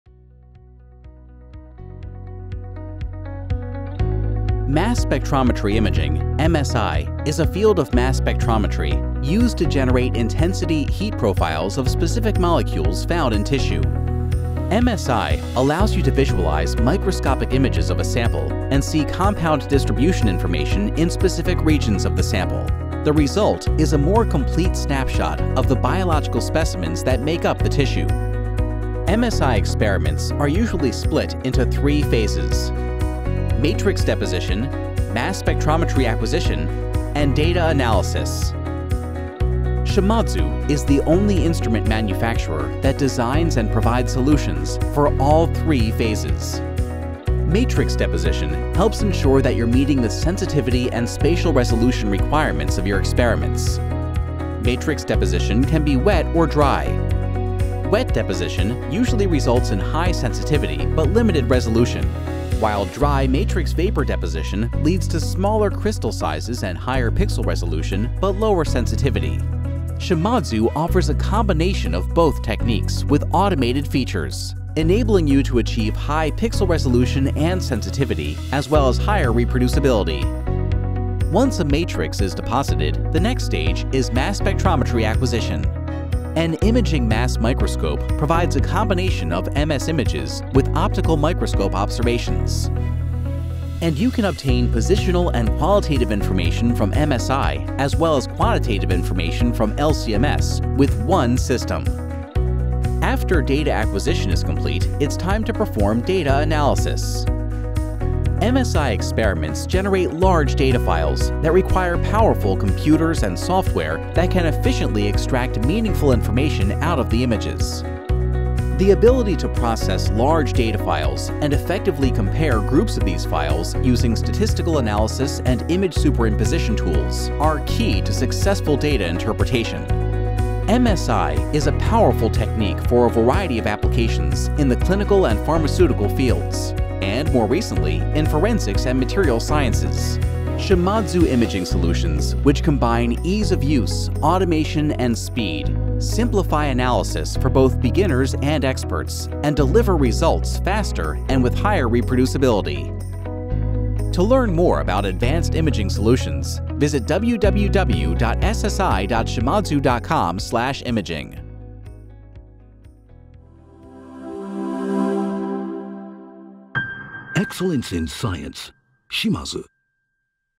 Introduction to Mass Spectrometry Imaging
This video provides a quick overview of mass spectrometry imaging (MSI), a field of mass spectrometry used to generate intensity/heat profiles of specific molecules found in tissue. MSI is a powerful technique for a variety of applications in the clinical and pharmaceutical fields and, more recently, in forensics and material sciences.

Shimadzu imaging solutions, which combine ease of use, automation and speed, simplify analysis for both beginners and experts and deliver results faster and with higher reproducibility.